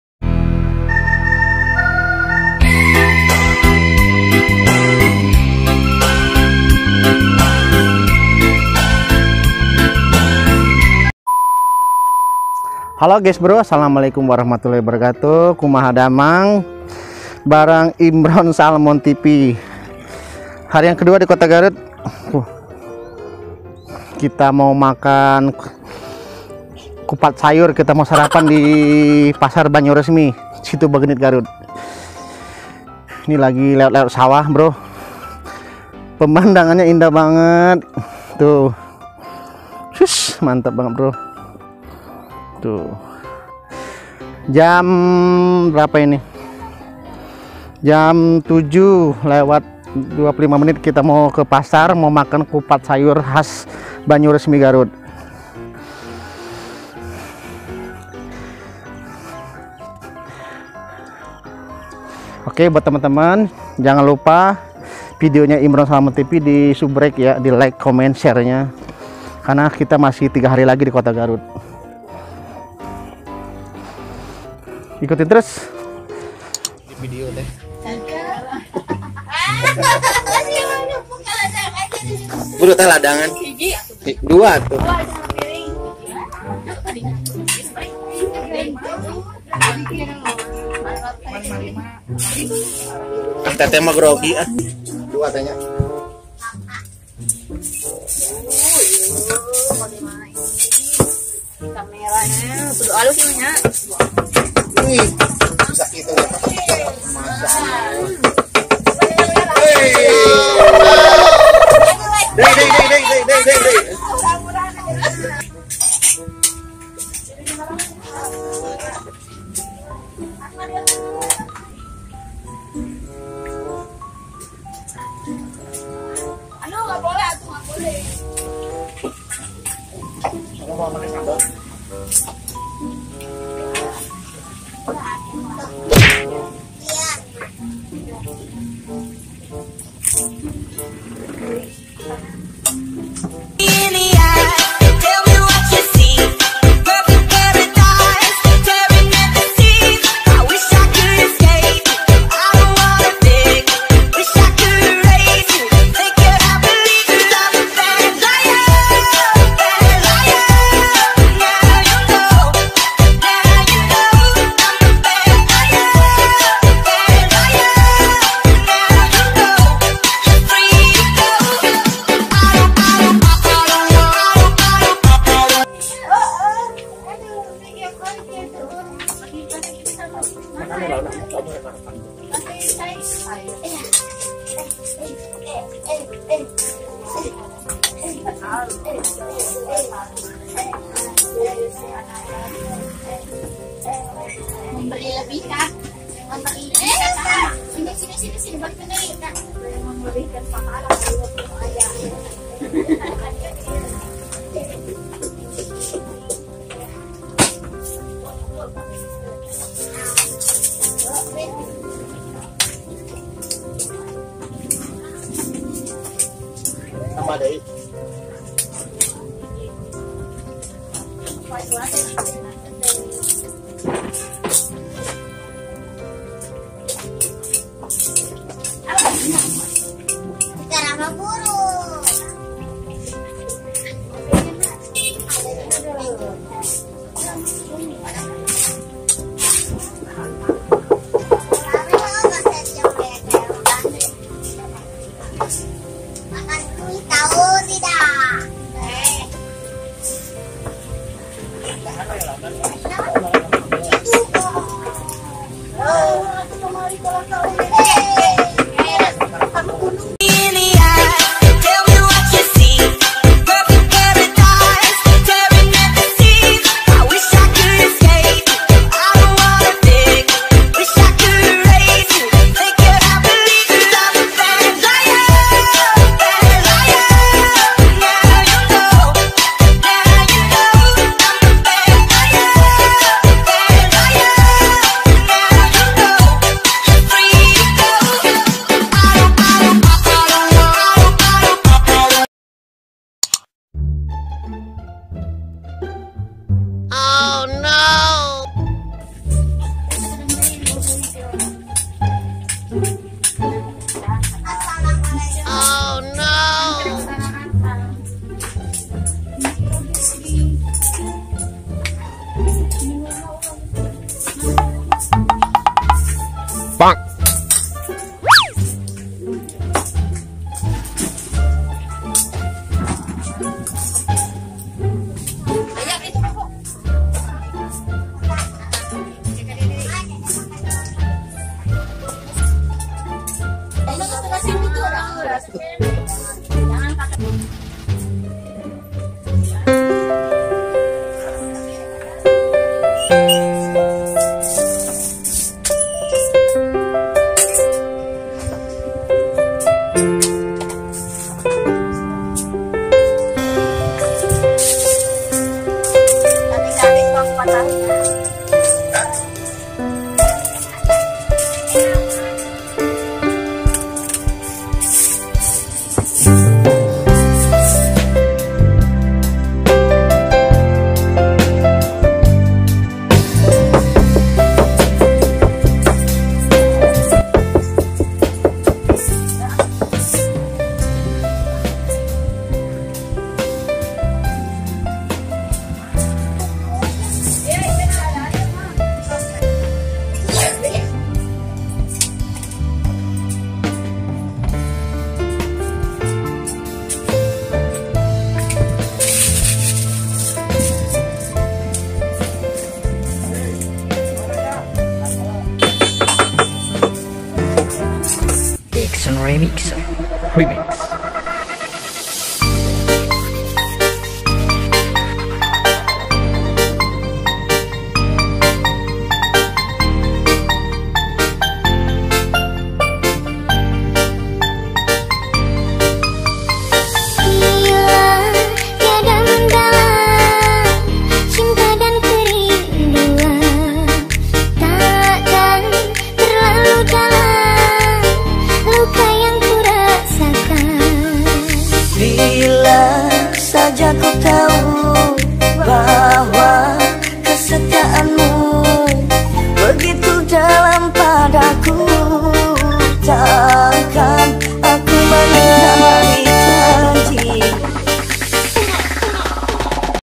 MAKAN KUPAT TAHU DI GARUT 😊 Bikin Pengen Balik Lagi Ke Garut
Terimah kasih buat semua Nya Subescriber Dan Teman2ku Semua Nya Atas Dukungan Dan Doa2 nya 😊😊😊

Lagi Di Garut Makan Kupat Tahu Di Pasar Banyuresmi Garut
Yg paling Enak Dan Top Markotop 😊😊😊

@Ria ricis
@Atta Halilintar
@Baim Paula
@Sule Channel
@Farida Nurhan
@Panji Petualang

Hatur nuhun buat Semua Nya